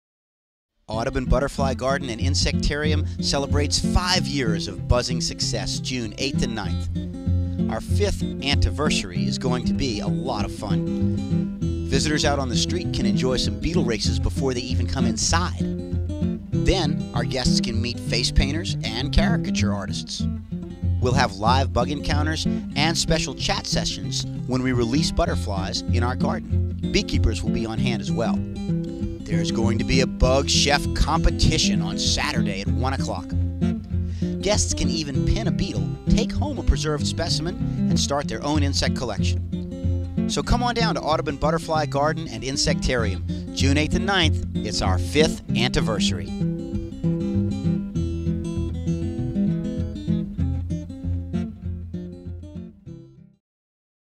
Celebrate the 5th Ant-iversary of Audubon Butterfly Garden and Insectarium!
Join the fun as we celebrate the 5th Ant-iversary of the Butterfly Garden and Insectarium the weekend of June 8 and 9, 2013. The party starts at 10am! See you then!

for more info about the 5th Ant-iversary event.